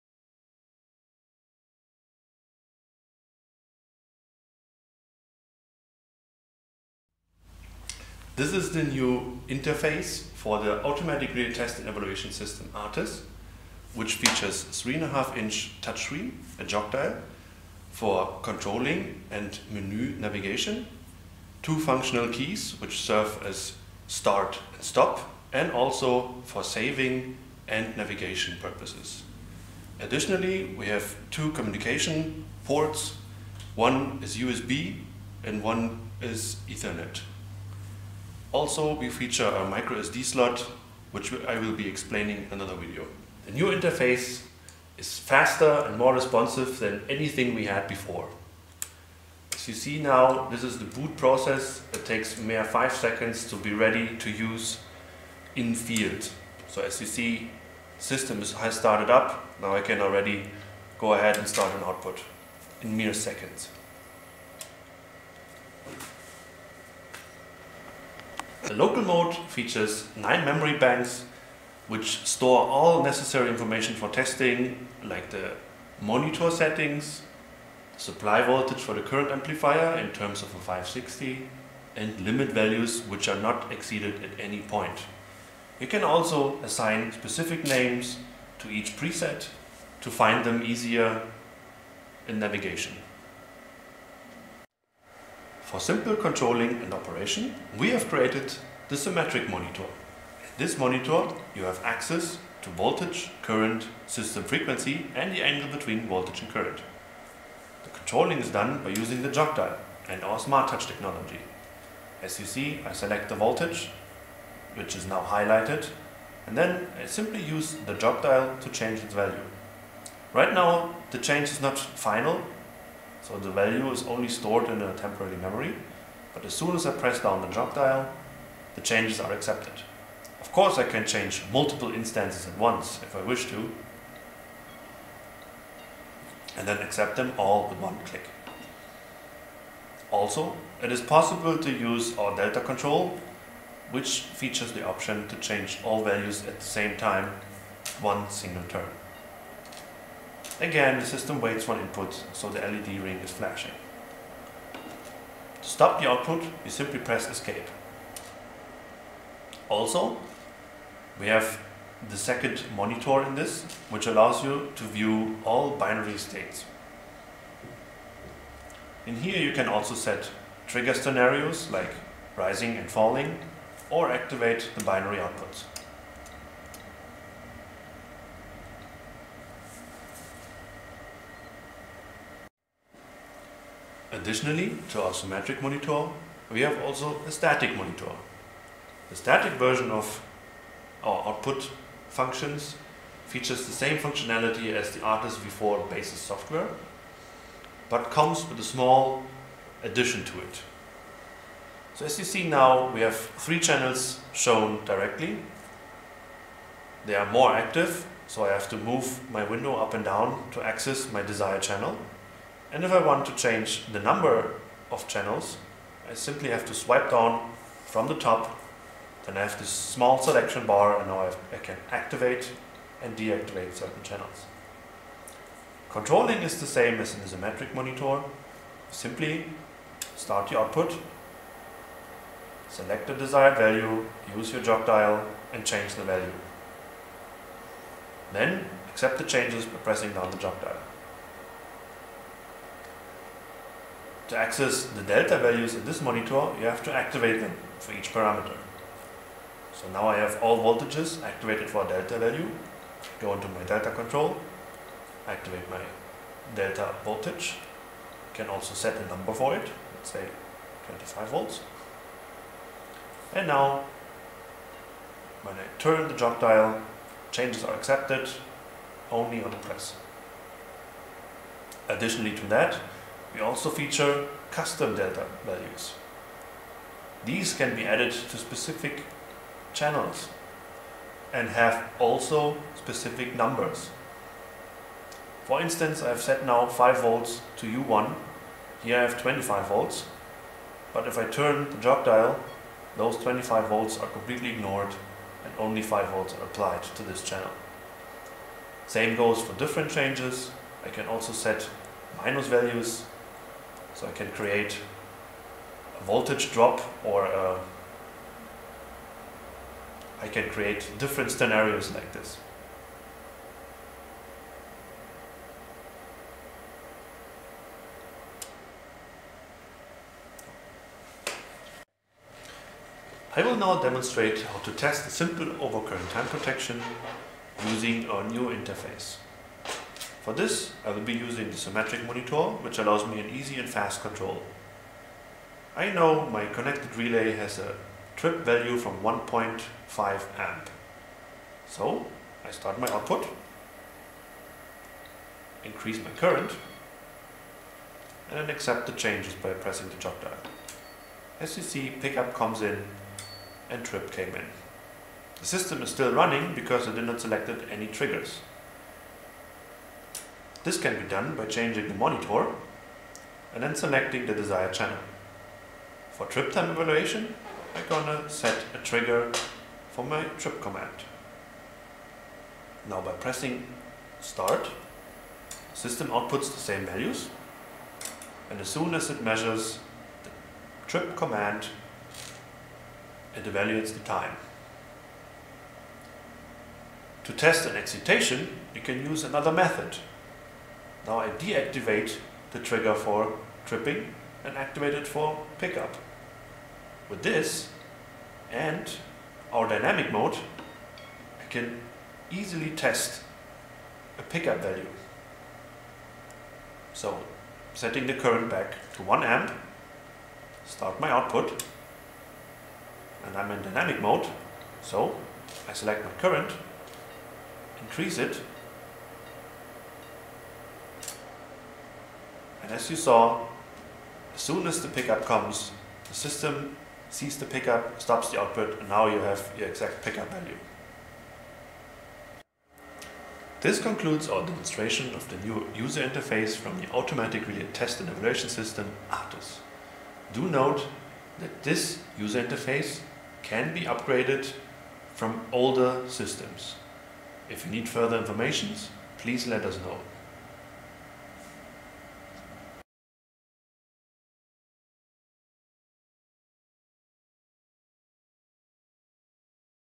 ARTES 460 | 560 - Automatic Relay Test Systems
ARTES 460 | 560 test systems are practical solutions which can cope with just about any relay testing task.